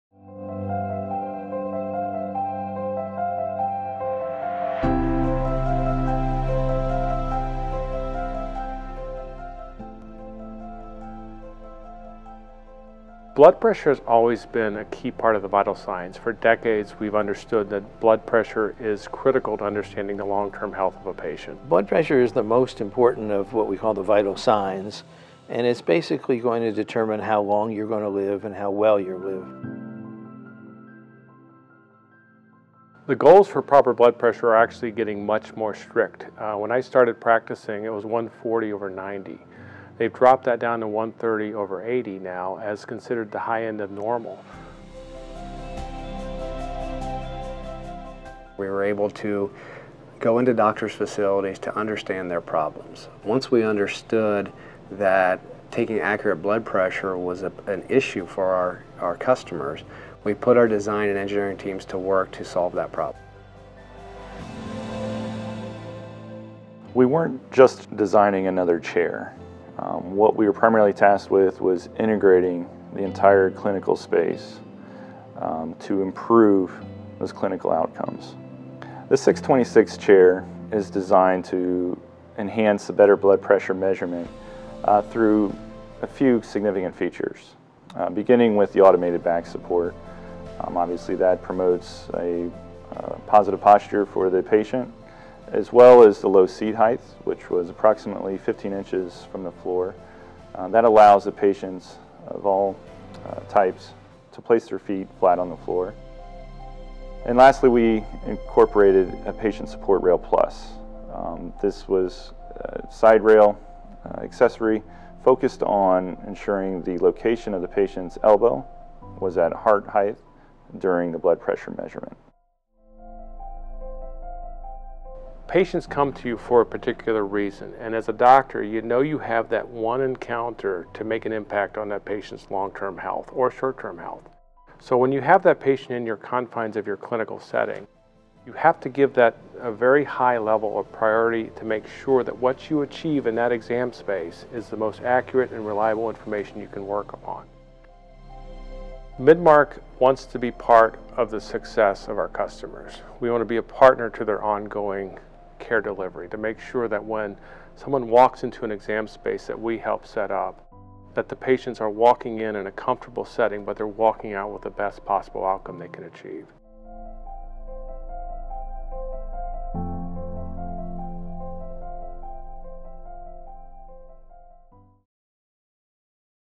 Behind the Design: Better BP
In this behind the scenes feature, we discuss the importance of accurate blood pressure measurements and how we're shaping the clinical space to enhance the healthcare experience.